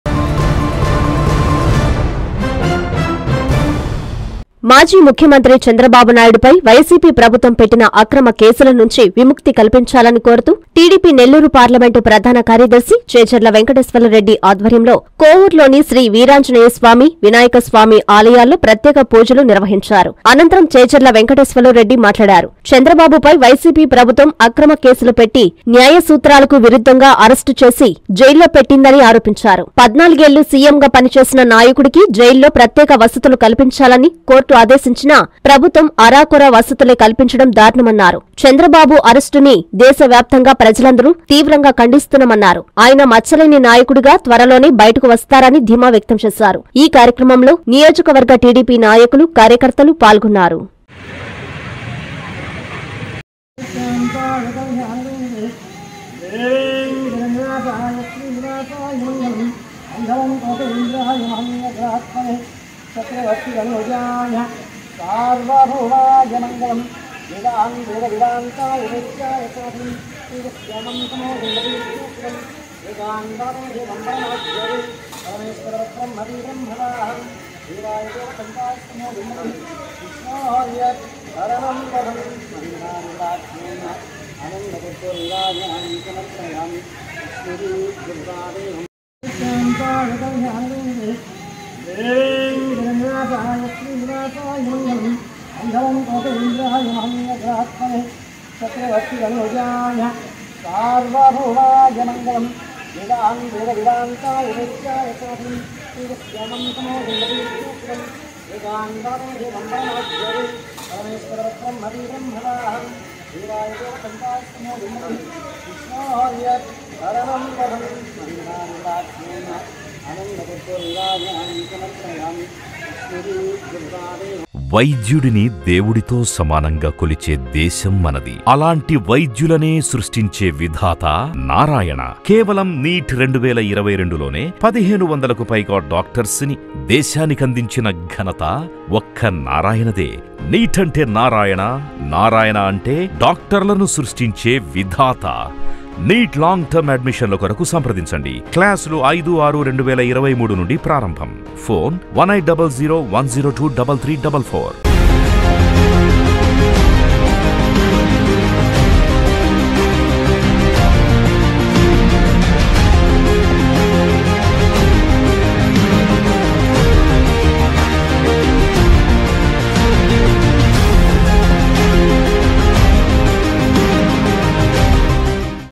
చంద్రబాబు అరెస్టు న్యాయ సూత్రాలకు విరుద్ధం | Special Pooja | Chandrababu Naidu Arrest | Kovuru
- కోవూరు వీరాంజ‌నేస్వామి ఆల‌యంలో చేజ‌ర్ల ప్ర‌త్యేక పూజ‌లు